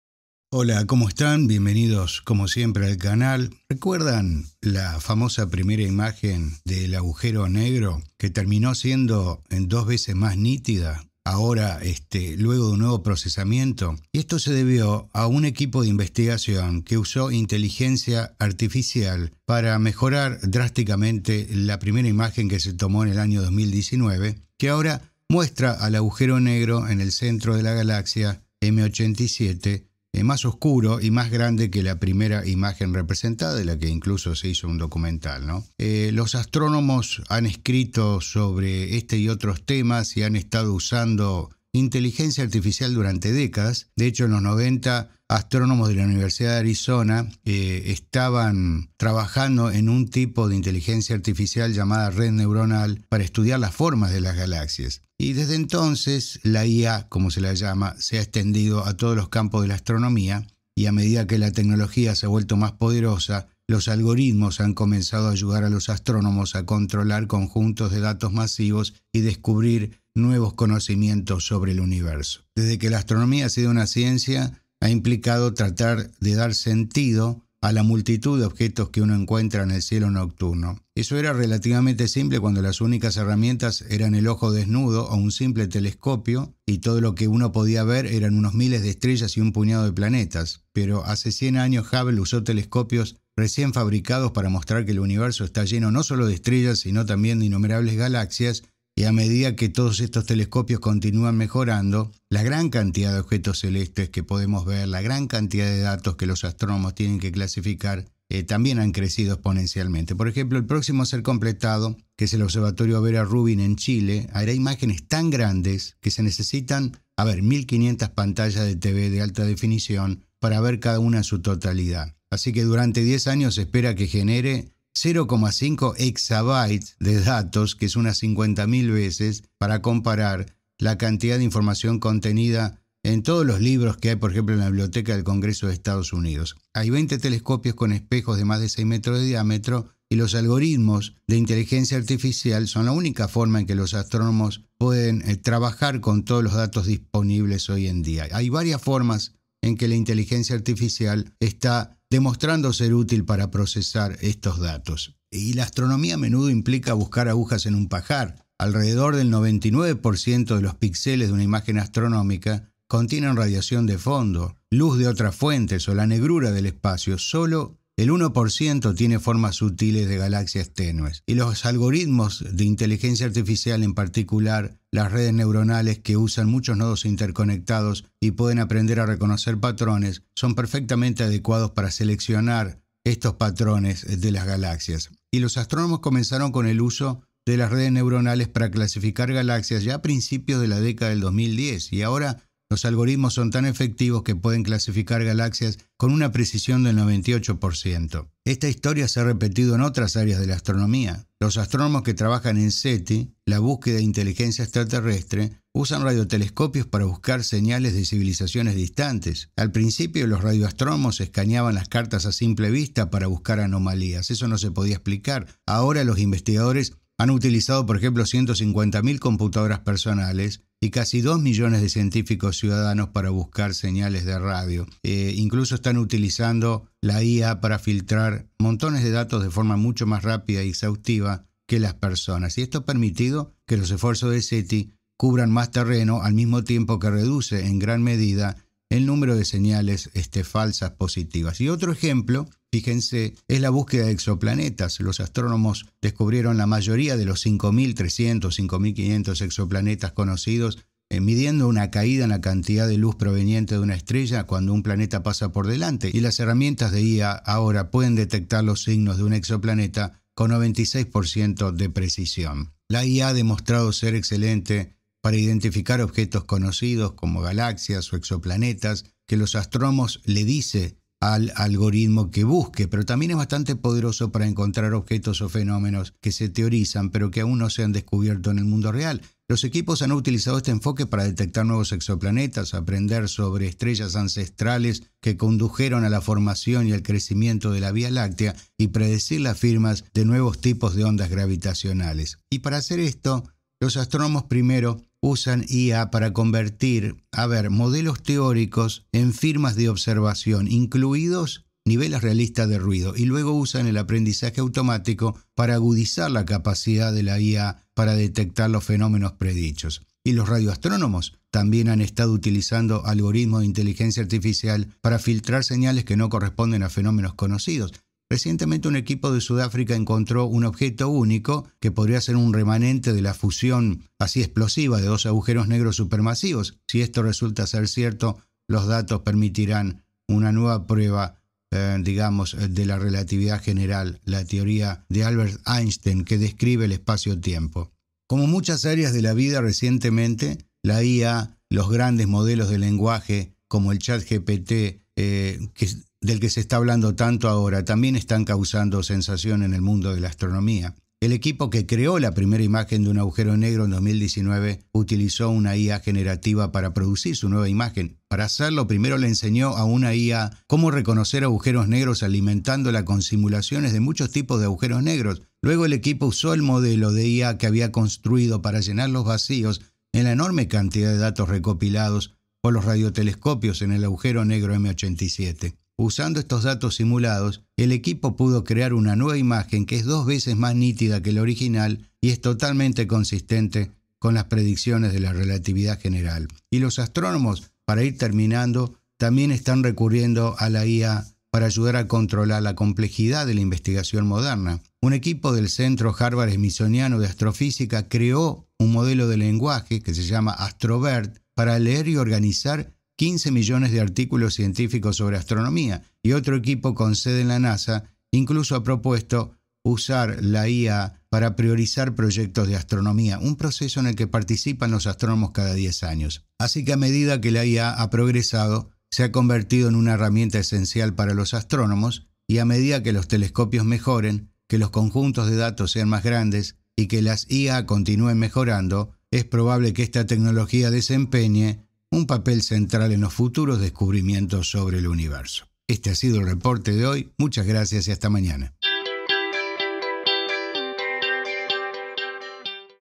!LA IA (INTELIGENCIA ARTIFICIAL) TAMBIEN ESTA REVOLUCIONANDO LA ASTRONOMIA!
La famosa primera imagen de un agujero negroacaba de ser dos veces más nítido. Un equipo de investigación usó inteligencia artificial para mejorar drásticamentesu primera imagende 2019, que ahora muestra el agujero negro en el centro de la galaxia M87 más oscuro y más grande que la primera imagen representada.